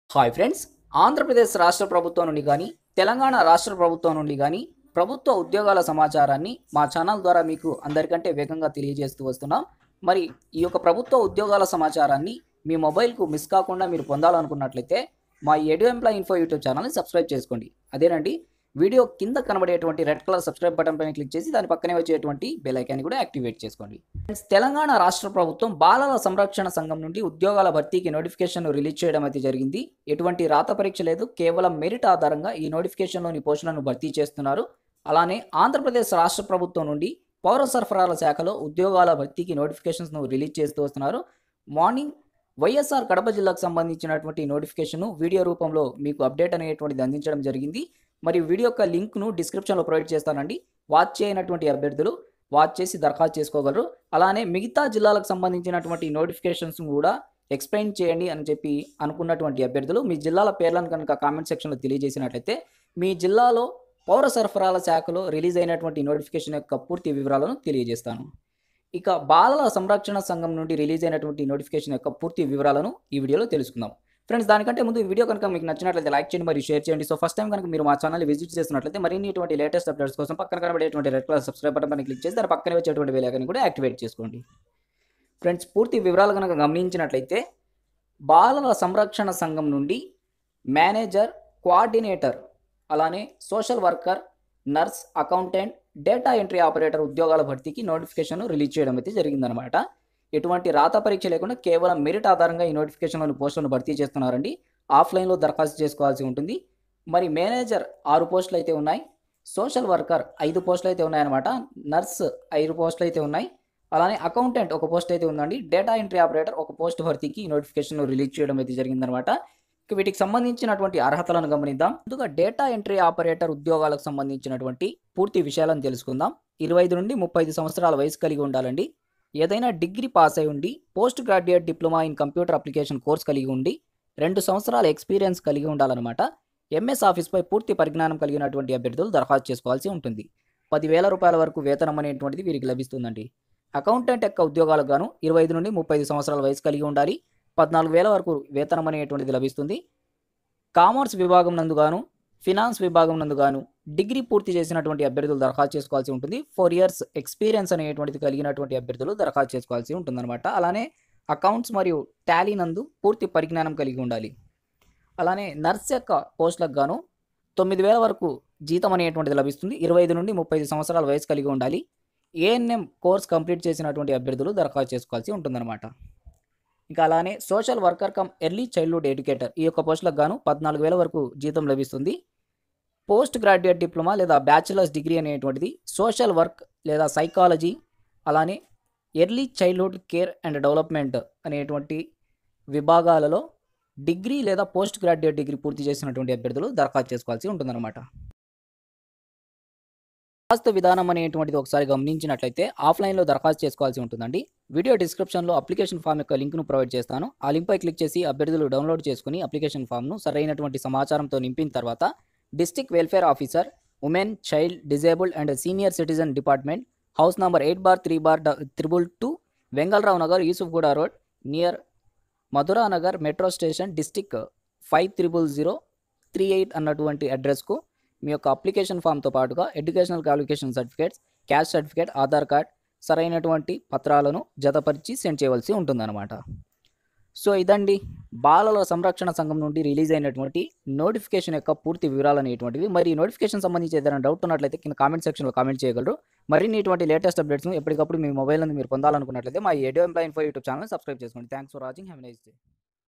పిల్లల సంరక్షణ సంఘంలో ఉద్యోగాలు భర్తీ | Child Protection Society Recruitment 2020
This video explains District Child protection Society recruitment 2020 details telugu.